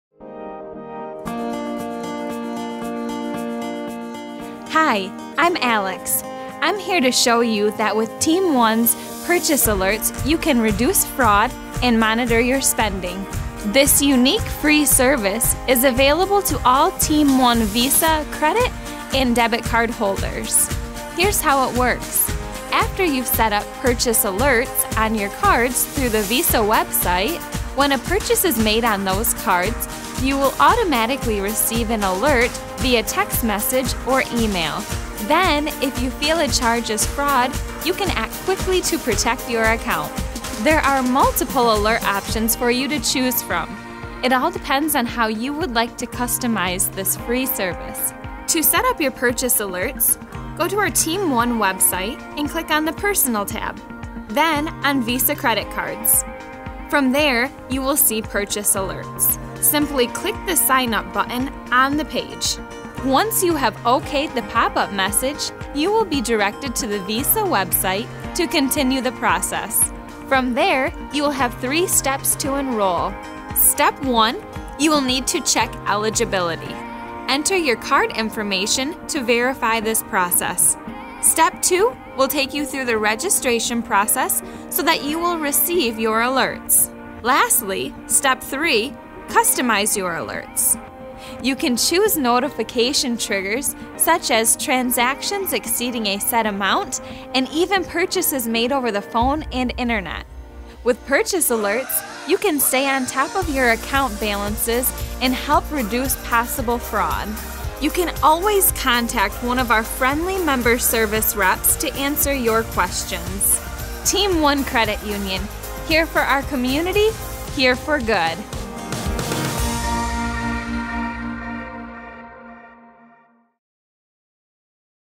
Team One Purchase Alerts
With near real-time Purchase Alerts from Team One powered by Visa, you can receive updates on your Visa® card activity. And this unique service is available free for Team One Visa Credit Card and Debit Cardholders.